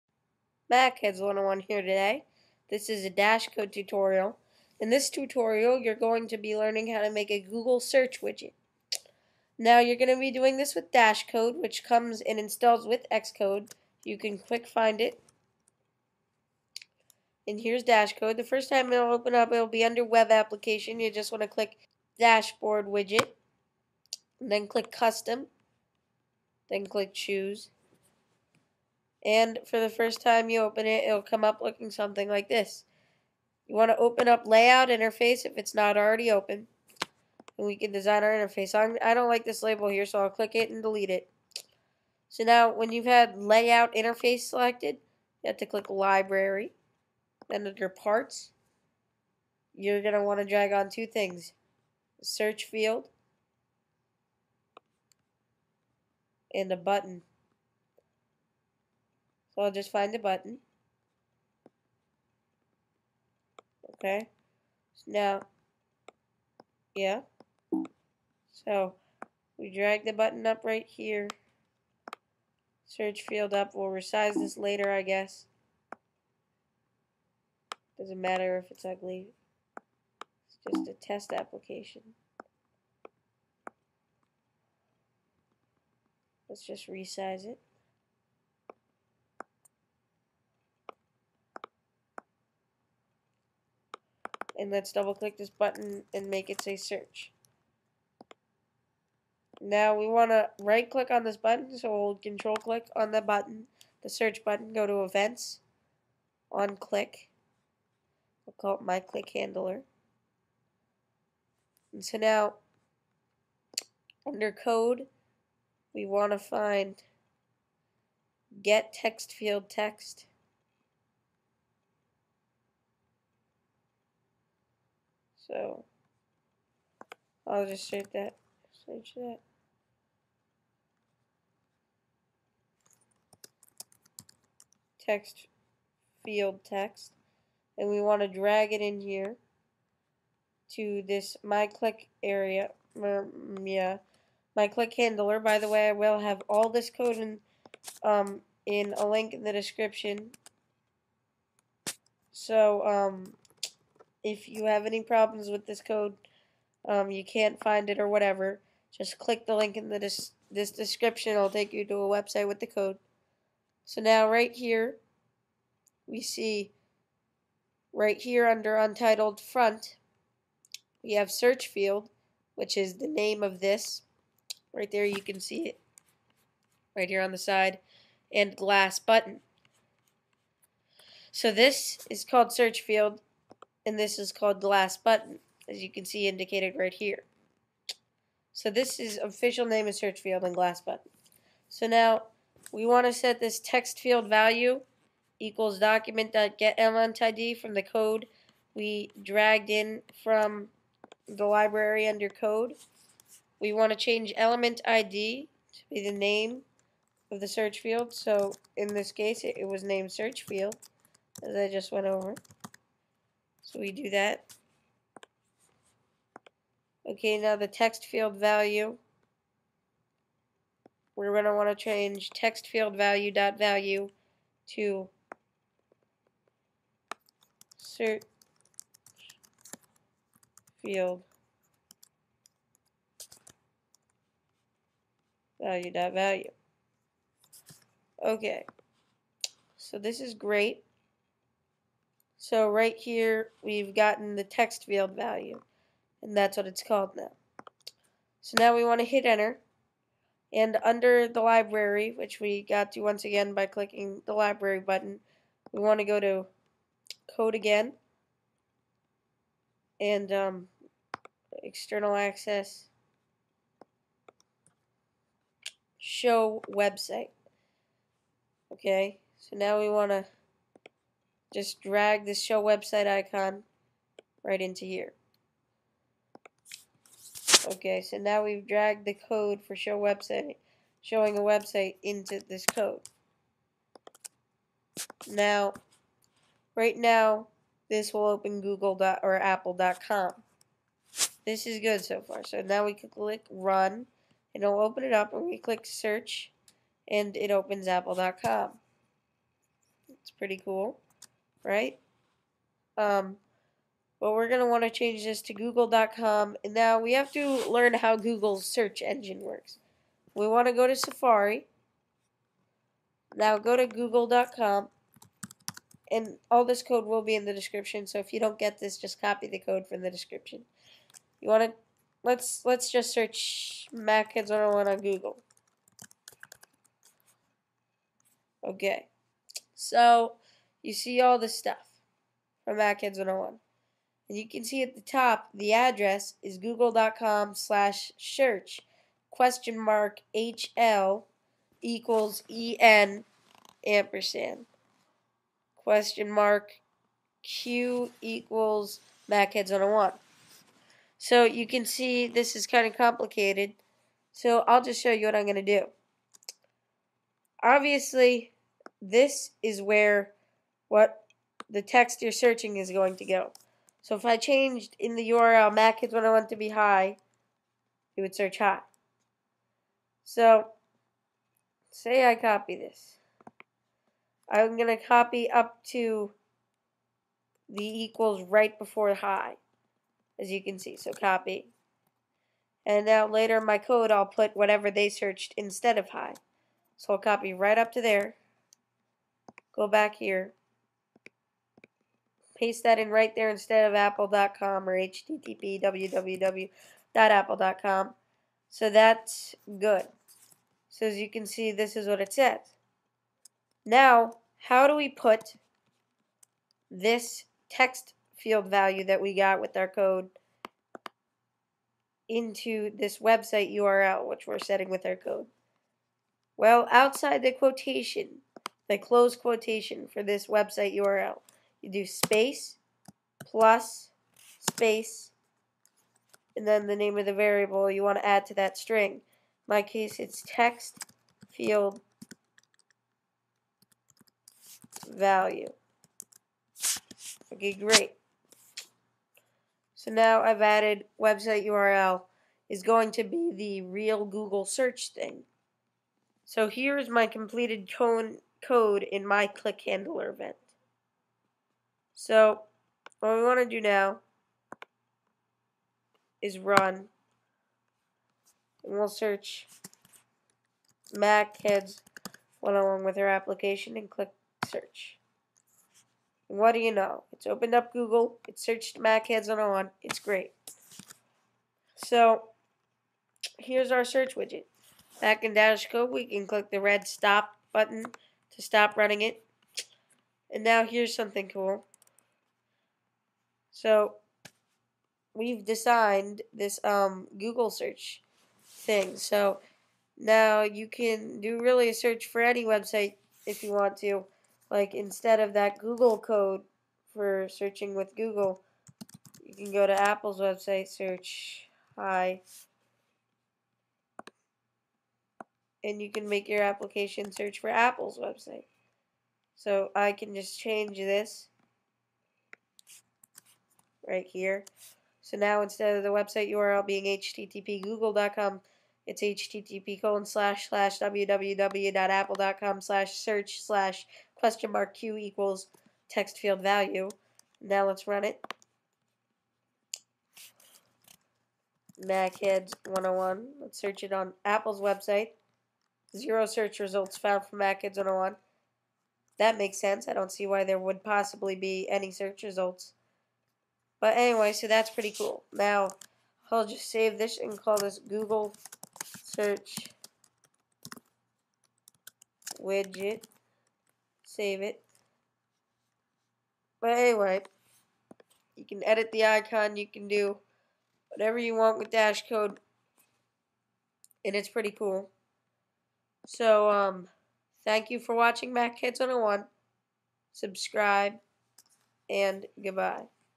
Making a Search Widget With Dashcode
I show you how to make a simple widget.

Here is a link to download the apple search widget